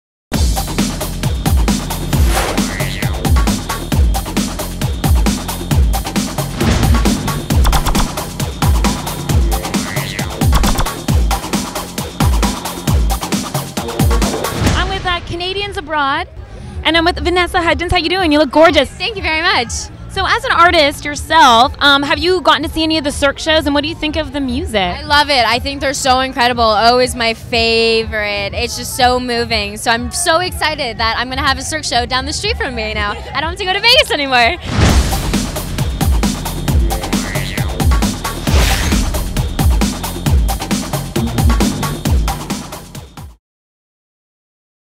Vanessa Hudgens Interview for the Cirque du Soleil Premiere of Iris in Hollywood
Canadians Abroad interviews Vanessa Hudgens at Cirque du Soleil IRIS in Hollywood, September 25, 2011.